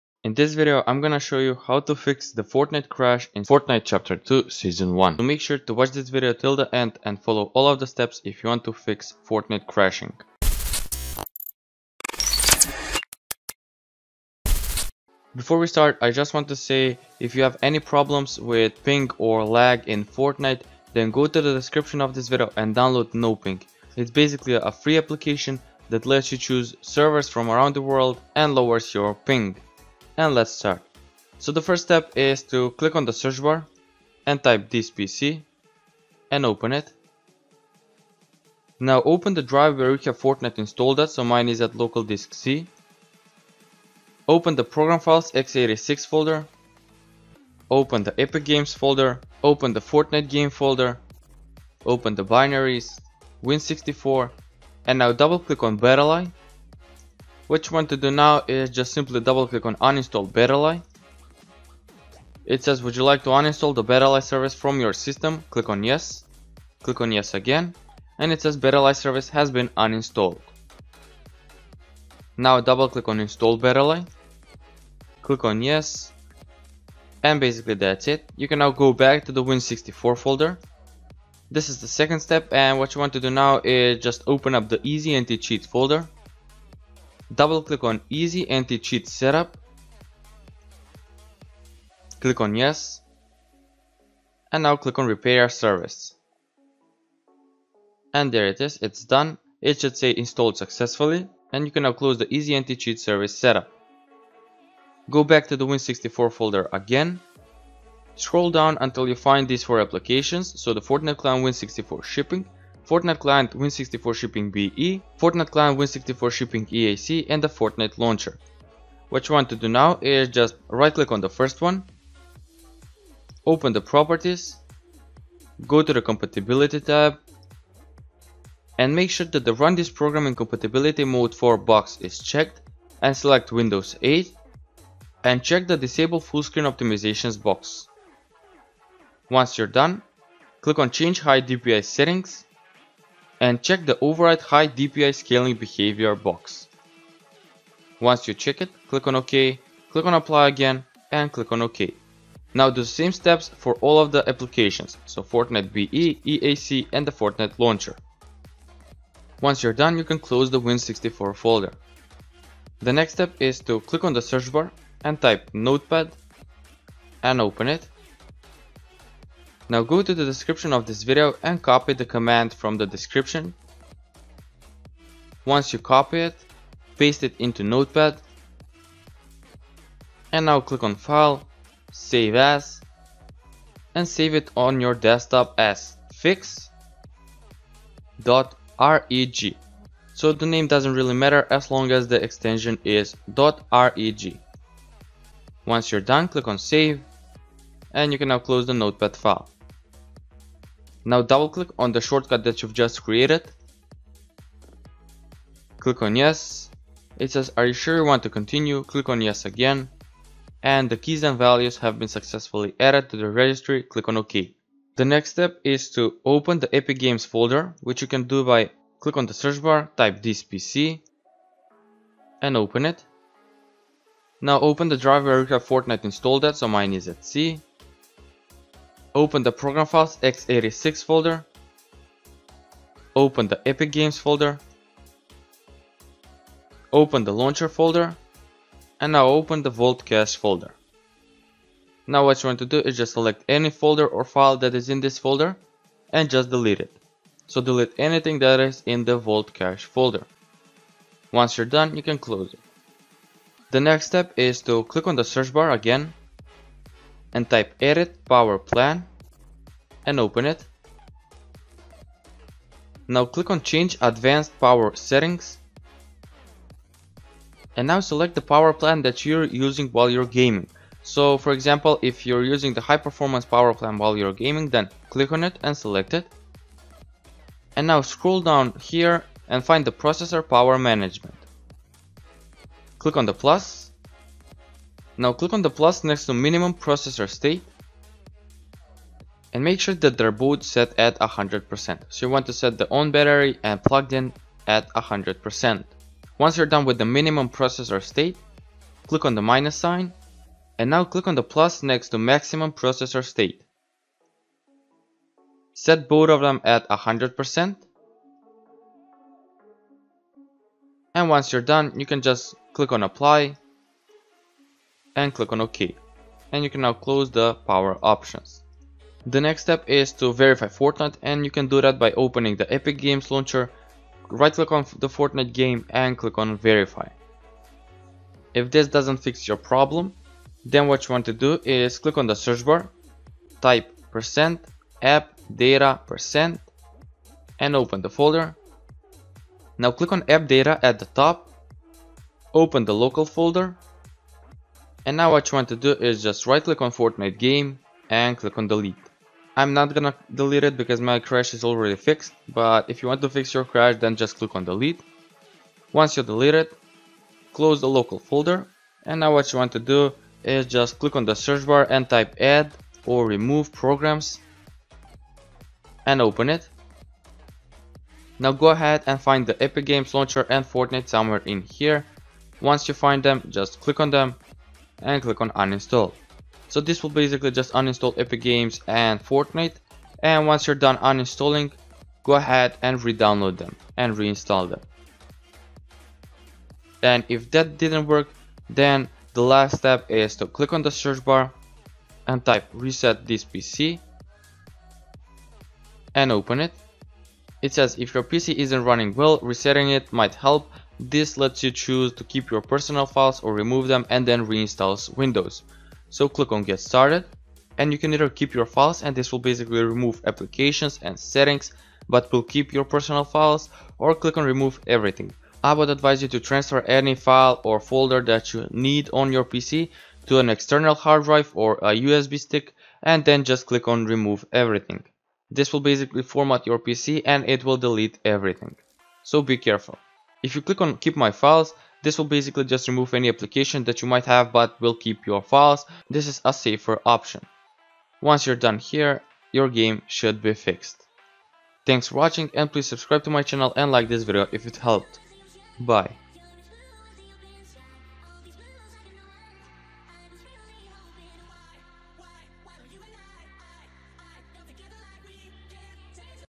FIX FORTNITE CRASH SEASON 11 ( FIX FORTNITE CRASHING ERROR CHAPTER 2 )
In this video, I show you how to fix the Fortnite crash in Fortnite Chapter 2 Season 1 or the Fortnite Season 11. Fix the Fortnite crashing error in the new Chapter 2 Season 1. Fix the Fortnite application has crashed and will now close error and fix the Fortnite Not Opening crash in Fortnite Chapter 2 Season 1 or Fortnite Season 11.

[HKEY_LOCAL_MACHINE\SYSTEM\CurrentControlSet\Control\GraphicsDrivers]

"TdrDelay"=hex(b):08,00,00,00,00,00,00,00
Lower your ping with NoPing.